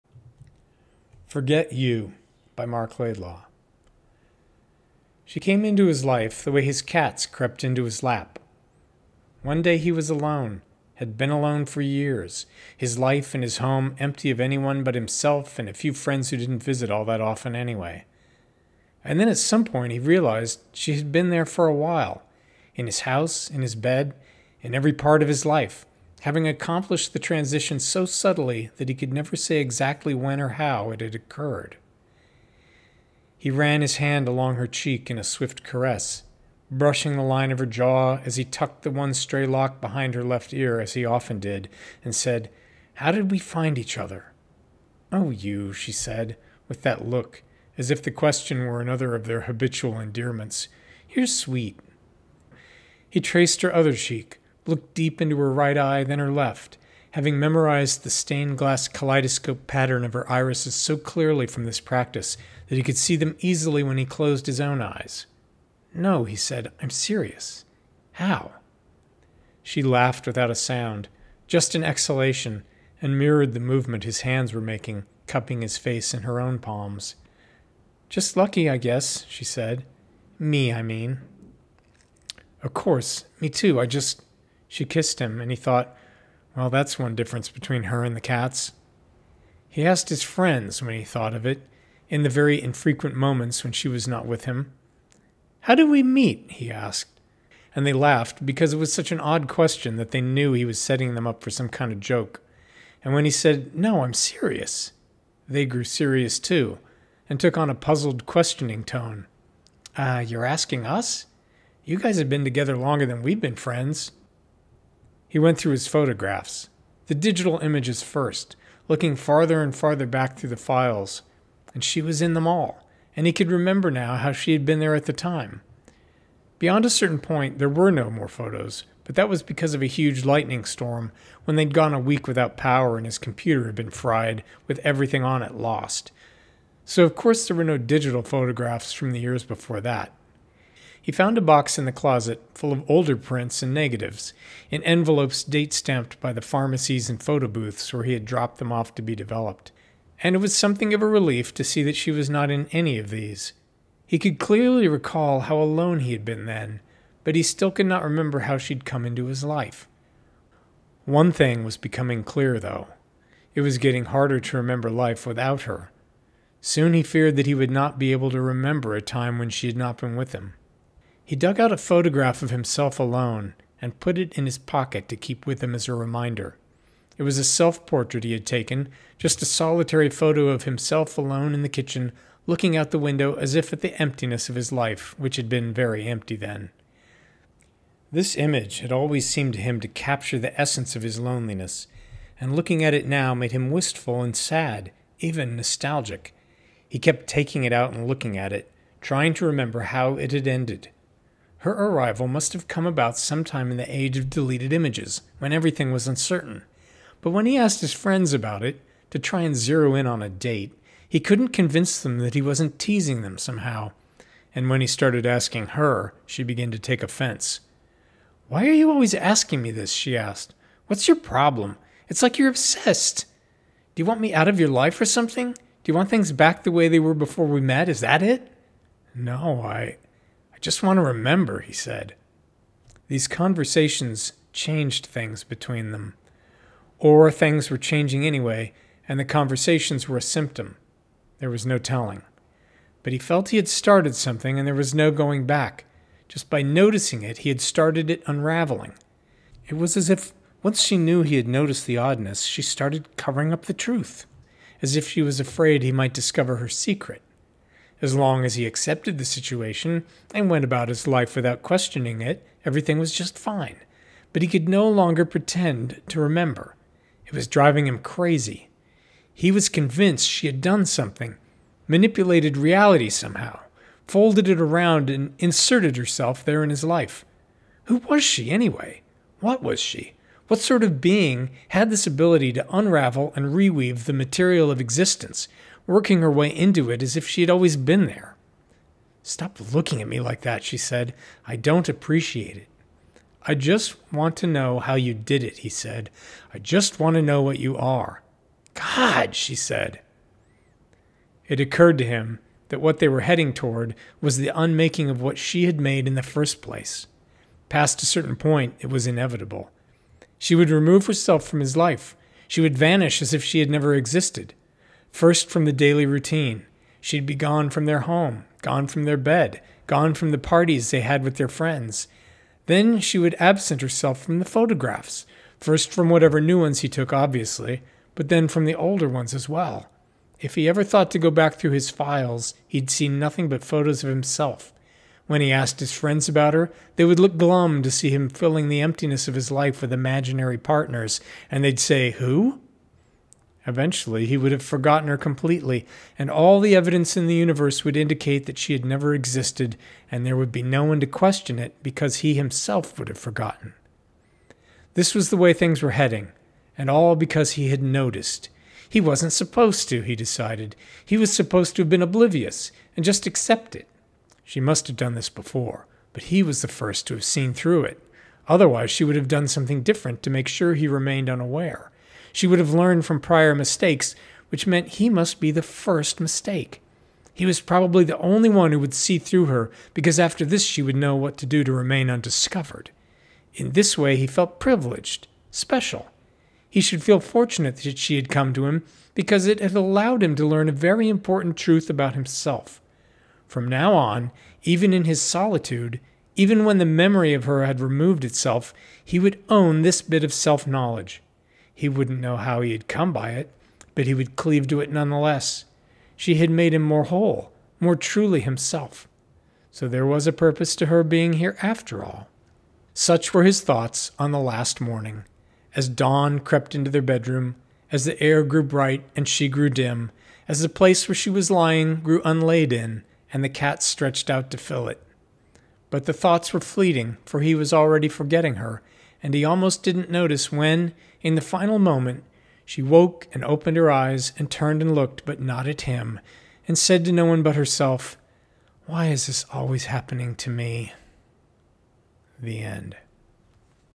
Forget You
A very short tale which begins and ends with cats.

Or find it in my Kindle collection: